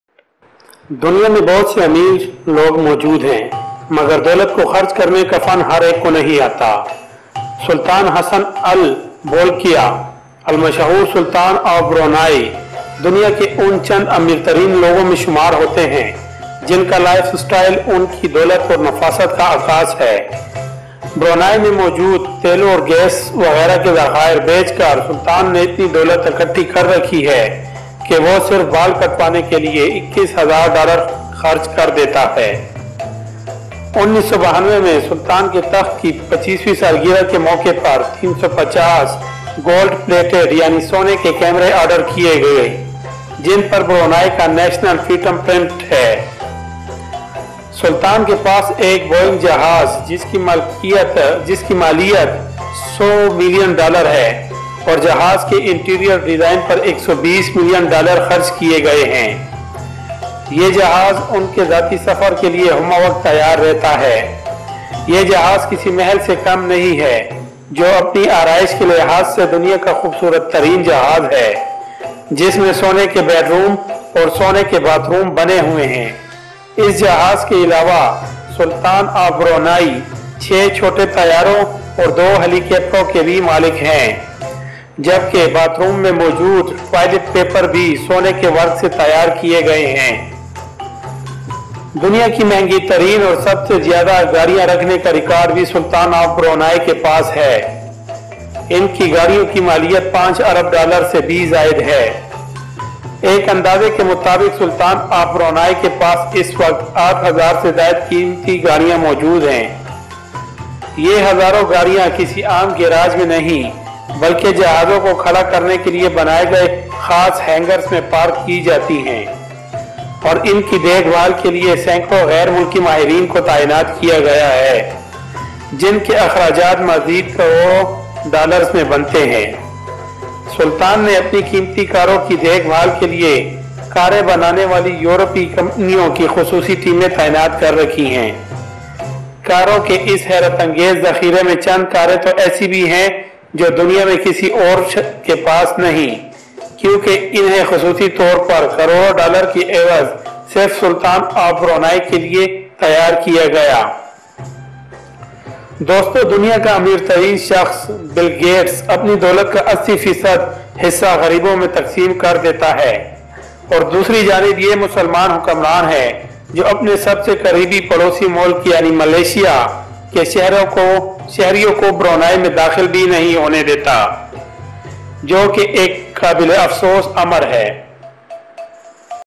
Sultan of Brunai Life Style 2018 in Urdu| Hindi
Luxury and Lavish Life Style of a Muslim king.